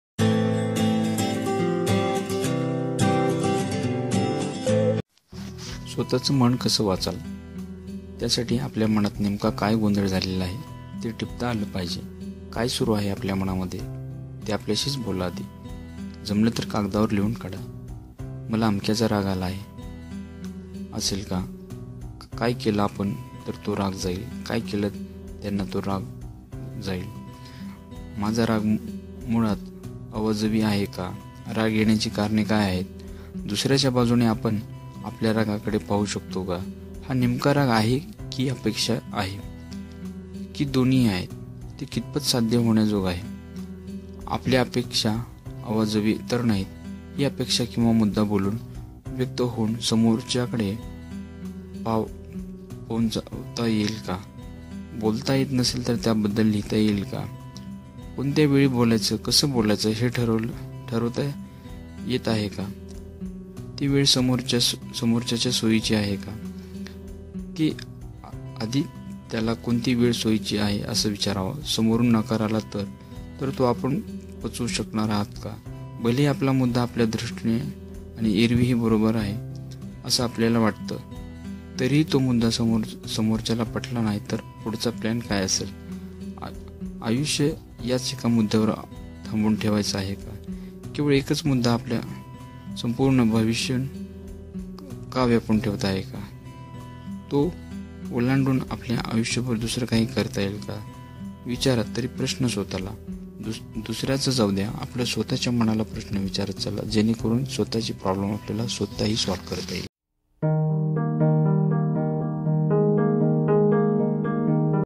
स्वतः च मन कस वाचाल ? How to Read Self Mind
When We thinking about us good and bad, that time first ask to own mind which is good and which is bad, so we have to shown from this video how to read own mind.